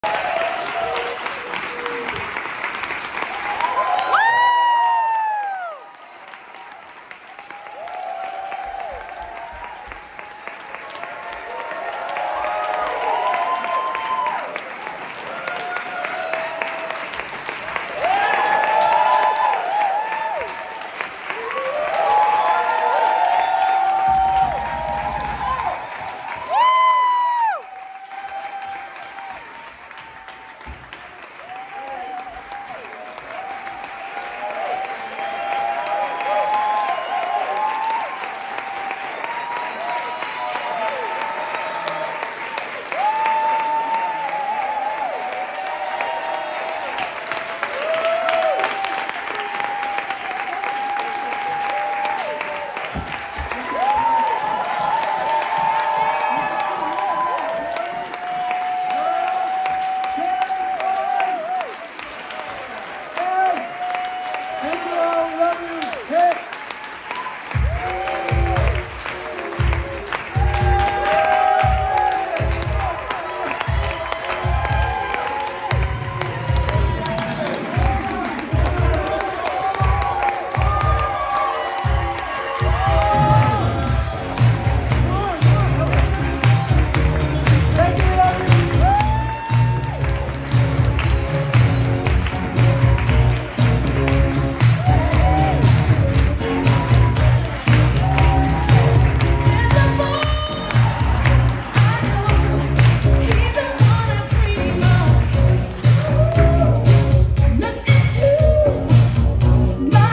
Walk On Through Gavin Creel - Off-Broadway FINAL SHOW Bows - MCC Theatre NYC - 1/7/24 @ 7PM
Bows from the final show of Walk On Through: Confessions of a Museum Novice featuring Gavin Creel.

MCC Theater Space - New York, NY
January 7, 2024 @ 7PM

@frayedbooks on Twitter/IG
and/or: